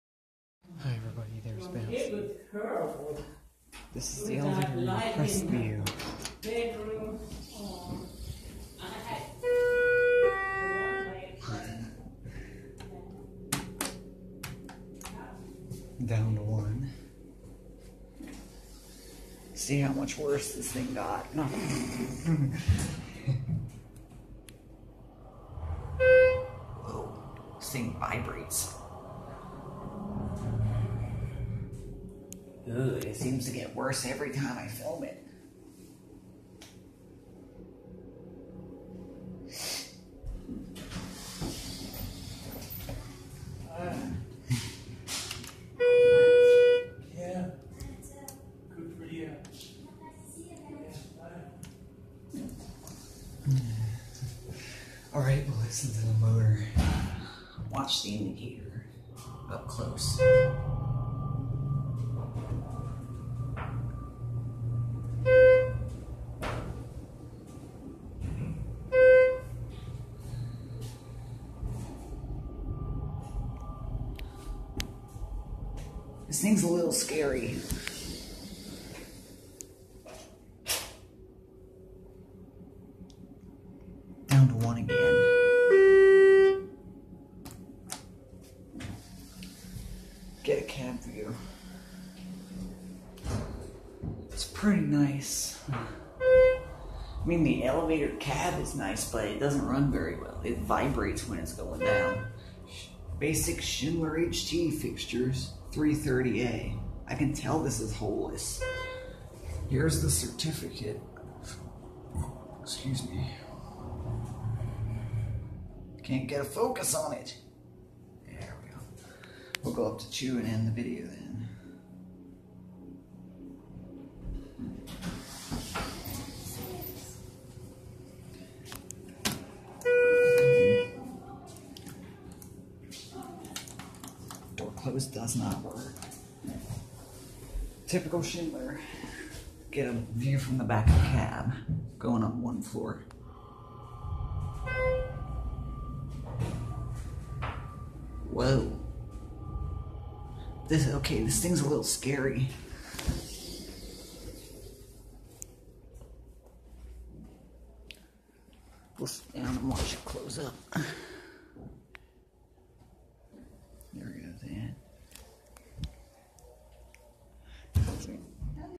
September 2018: Schindler 330A Hydraulic elevator @ Crest View Fairmount Homes Ephrata, PA
(taken 9/8/18) This is a retake of the crest view elevator at Fairmount. This elevator always seems to be getting worse and worse every time I film it. After all, it is a Schindler 330A.

Be sure to follow me MRM 906 loves elevators on Flickr, where I will post on the account at least once or twice a week.